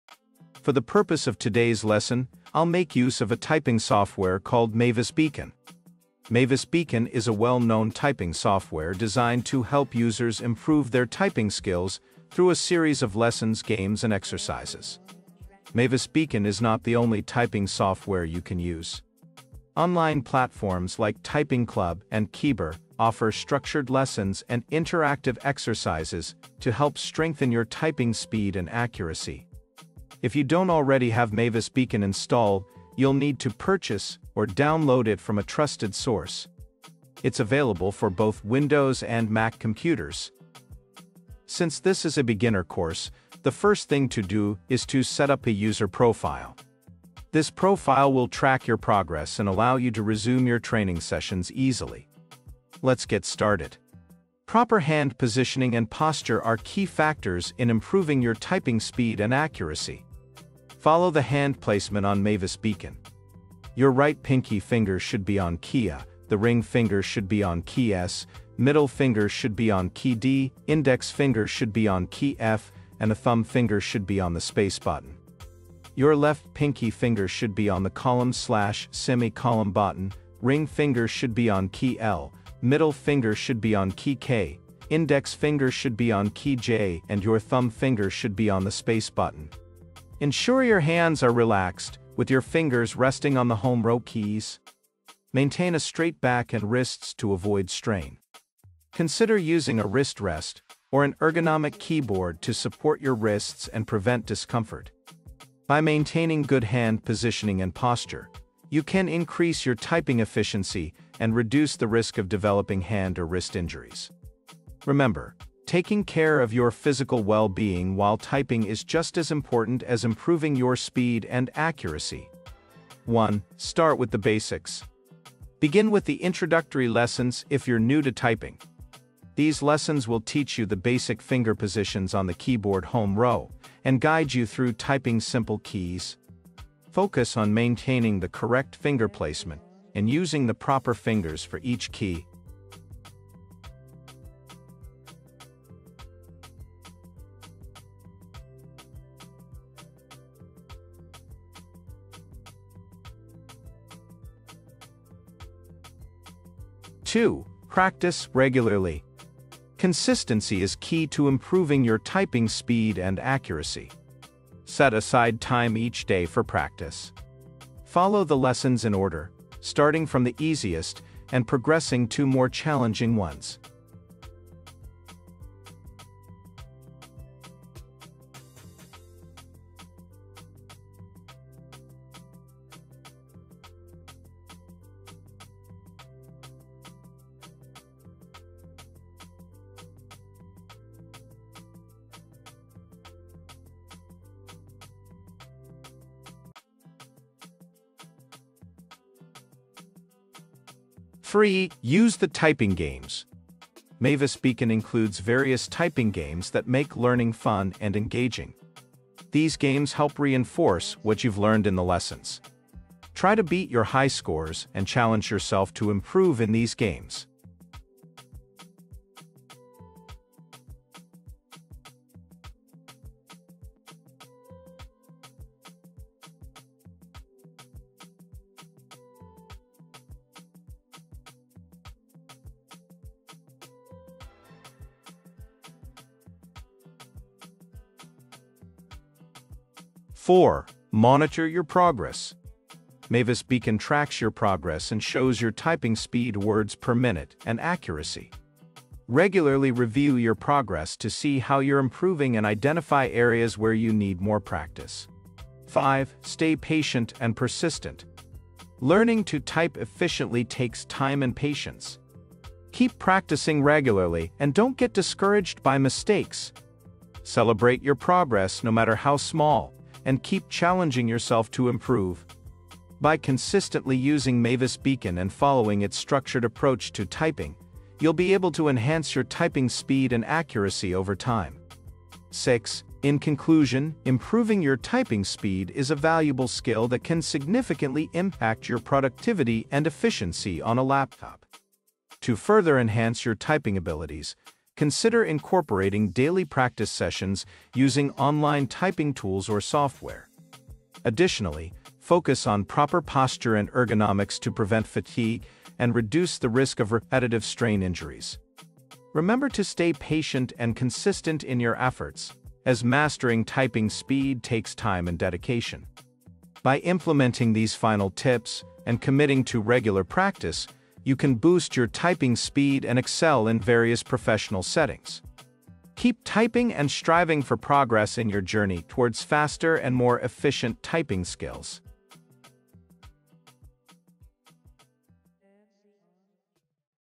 Praticed Typing on Computer Everyday and got really Fast. A Guide for Beginners 2024.
Learn how to type and type faster on a computer. This is a guide for beginners. In today's computer-driven world, typing has become a skill that is not only useful but also essential. Whether you are a student, professional, or simply someone who spends a lot of time on a laptop, knowing how to type can greatly enhance your productivity and efficiency. However, many people struggle with typing, especially beginners. That's why we have put together this comprehensive guide to help you learn how to type and type faster on a laptop. Whether you are brand new to typing or looking to improve your speed, this guide will provide you with the tools and techniques you need to become a proficient typist. So, let's get started and take your typing skills to the next level!

Typing quickly and accurately on a laptop is a crucial skill in today's digital age, especially for those who work in roles that require heavy computer use. Whether you're a student, professional, writer, or just someone who wants to boost their productivity, improving your typing speed can save you time and reduce the risk of repetitive strain injuries. In this video, we'll cover the basic techniques and tools you can use to type faster on a laptop, making your work more efficient and enjoyable. Let's dive in and start mastering the keyboard!